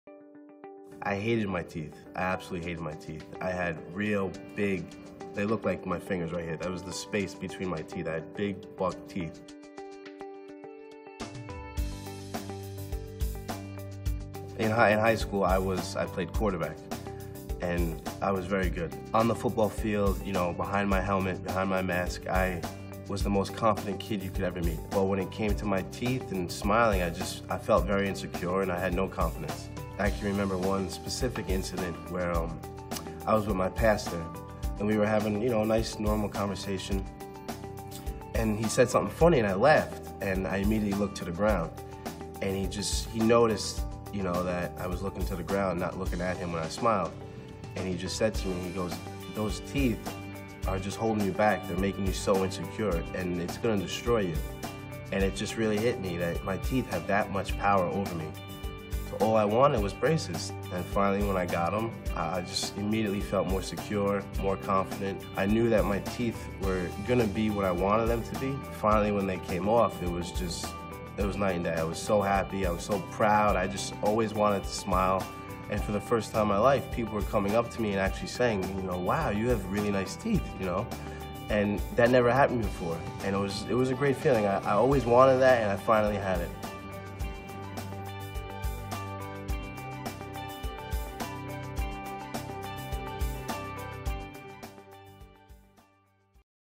Robert Gets His Confidence Back
Never let your teeth get in the way of your confidence. Our GEDC dentists and orthodontists are here to give you a smile that you love.